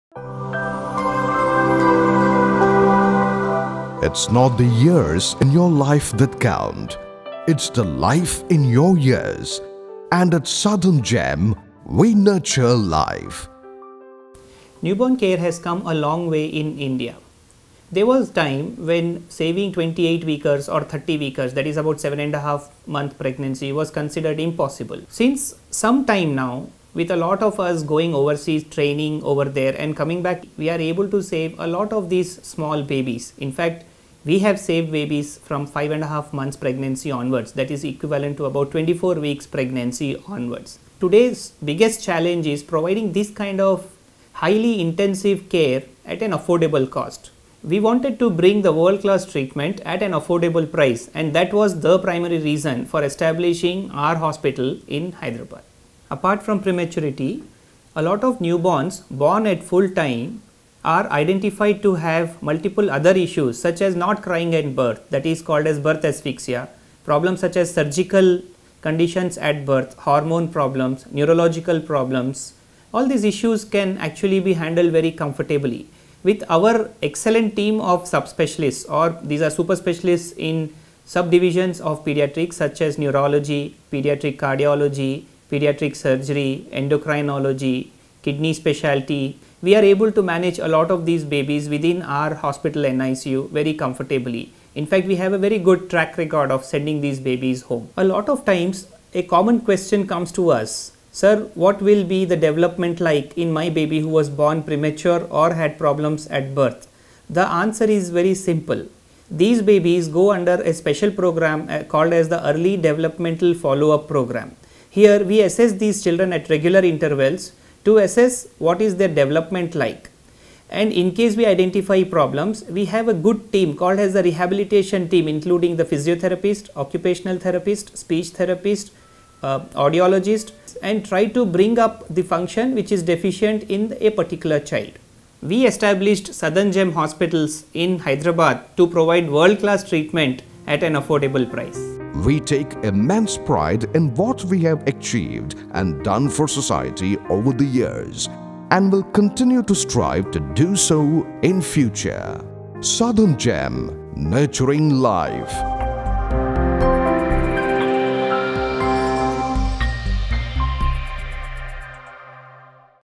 Newborn ICU (NICU) at Southern Gem Hospitals, Hyderabad - Dr Nitesh Agarwal
Newborn babies need highly specialized care in Newborn ICU (NICU) when they are born early (preterm/premature), have surgical problems such as CDH / TOF / Hernias, do not cry at birth (Birth Asphyxia), have an infection (Sepsis), have feed intolerance among other concerns. The Neonatology and Pediatrics team at Southern Gem Hospital is headed by Dr. Nitesh Agarwal. With rich experience in handling these babies, he has built a reputation for excellent professional caring in the field of neonatology not only in Hyderabad but also across India and abroad. Here he explains the reasons for establishing his NICU at  Southern Gem Hospital and how they have successfully met the highest expectations of many couples and provided the finest of experience for the mother and baby.